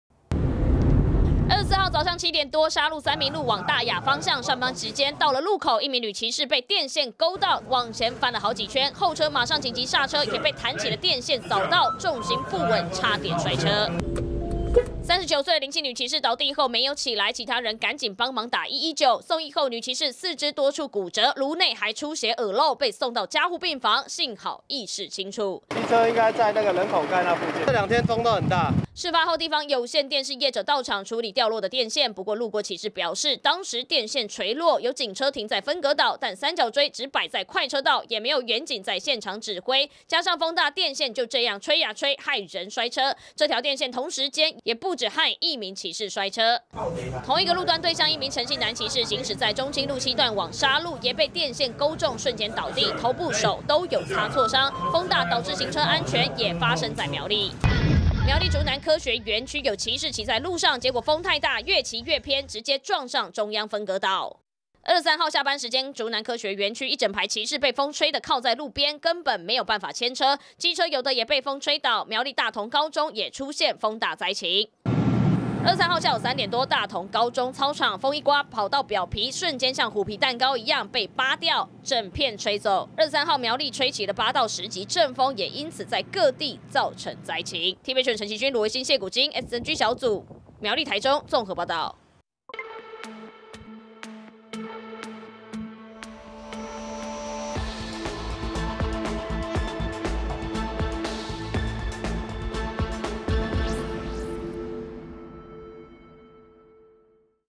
強風吹落電線 害女騎士慘摔顱內出血送醫｜TVBS新聞
東北季風發威，新竹苗栗各地都出現8~10級強陣風，不但有學校操場跑道被吹到飛起來，還有科學園區外因為風太大，整排機車都動彈不得只能停在旁邊，而在台中沙鹿，還有電線因為被強風吹落，害得女騎士經過不慎勾到摔出，顱內出血送醫搶救。